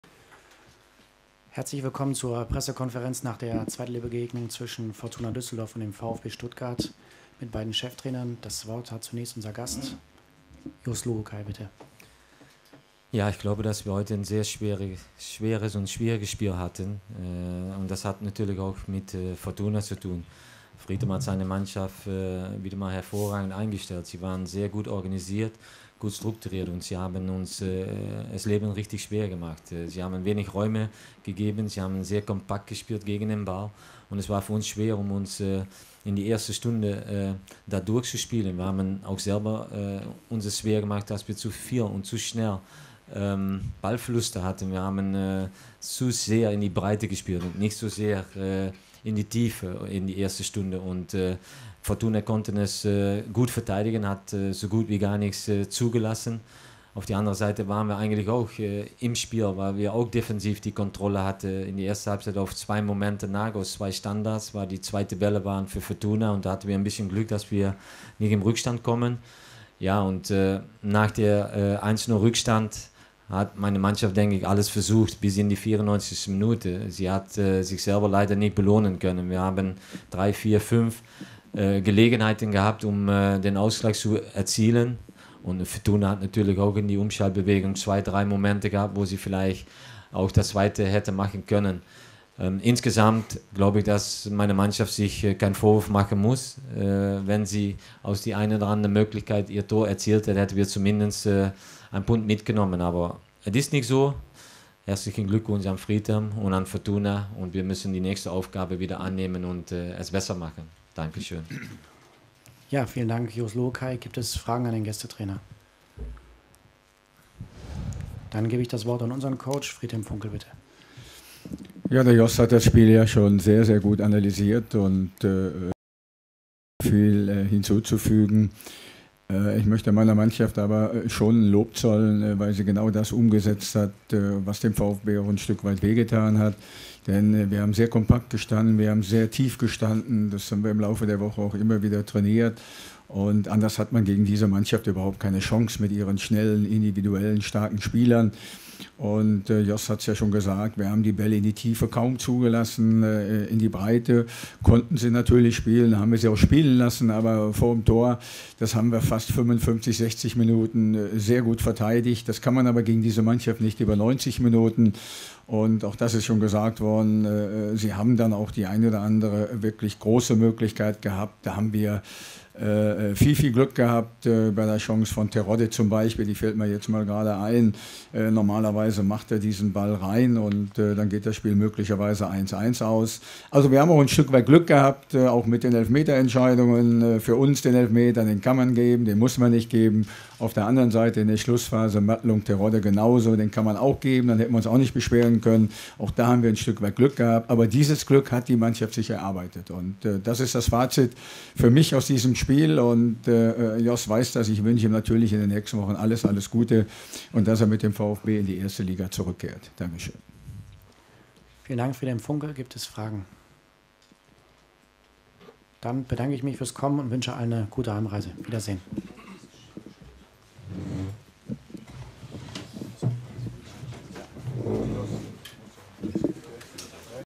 Pressekonferenz Fortuna - Vfb Stuttgart - 13.08.2016
Die Pressekonferenz vom Spiel
Fortuna Düsseldorf gegen Fvb Stuttgart am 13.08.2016,
mit dem Endergebnis 1:0. Saison 16/17 - Spieltag 2